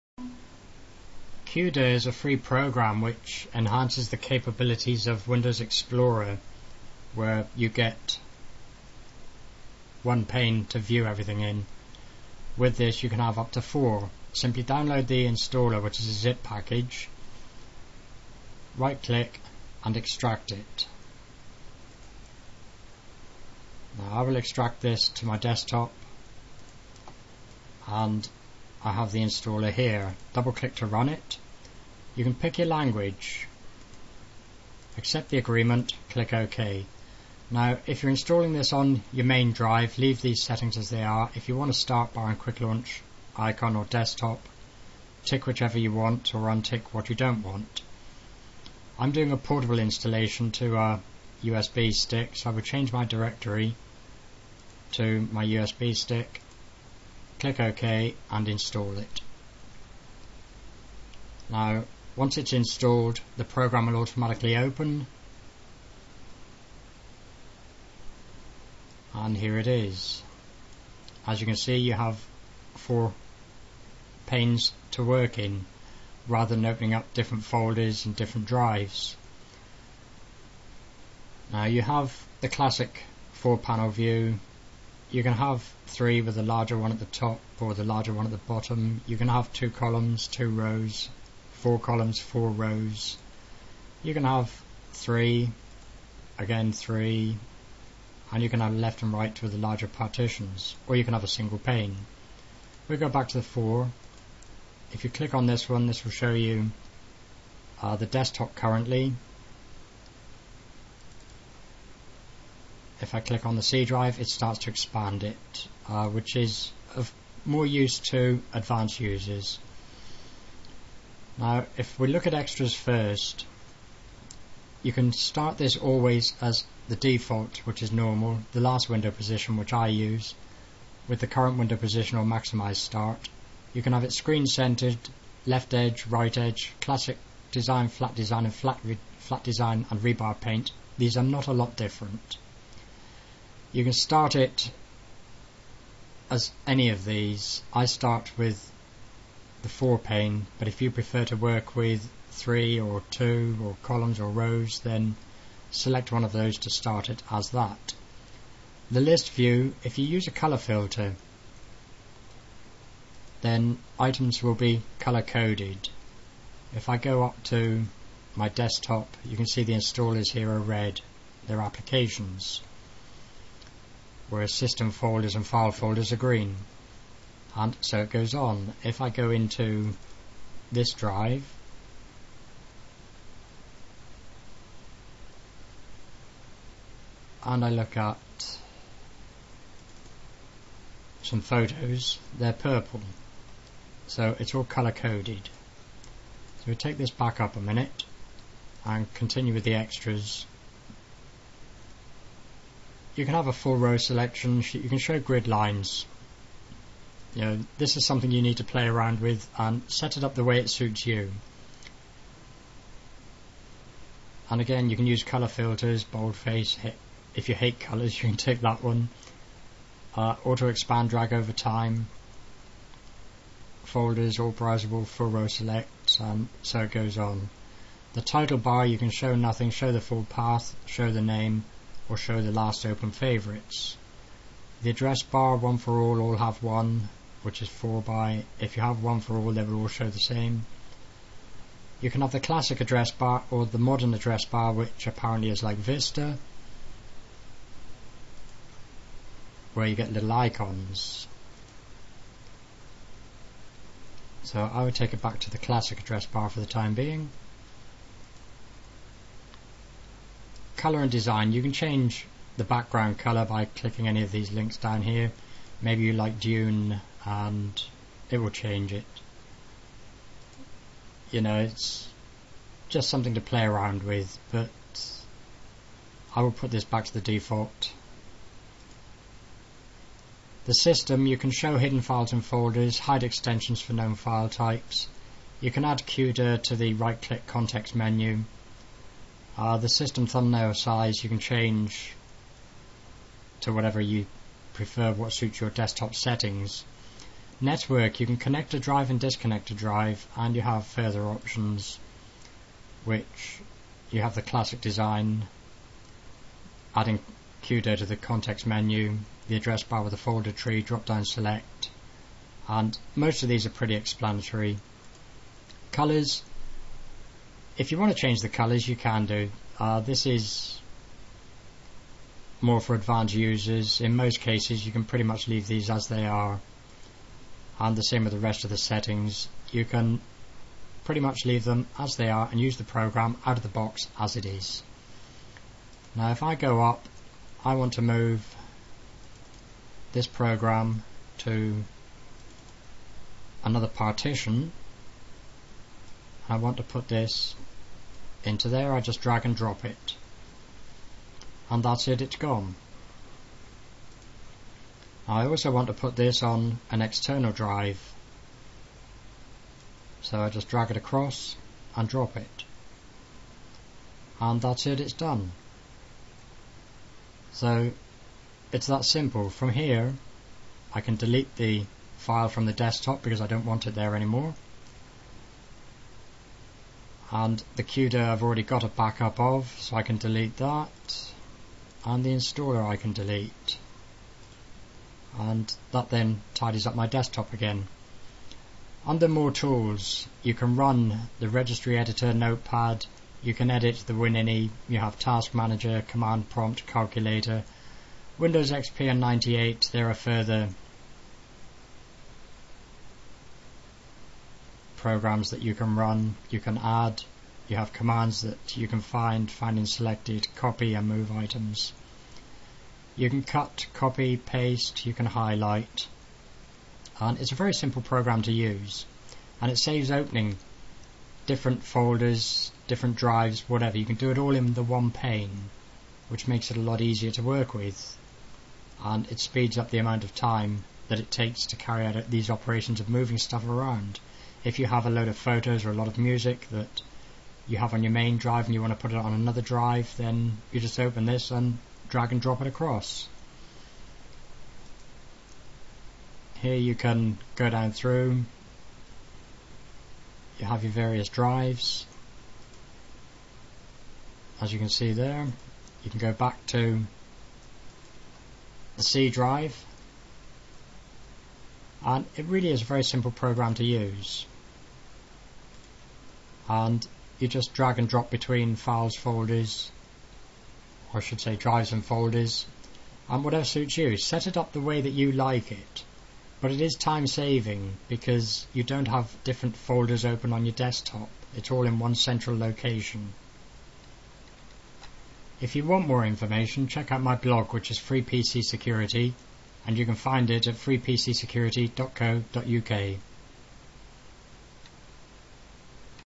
Q-Dir Free File Manager
Q-Dir is a free and easy to use piece of software which can be run from a hard drive or a USB stick.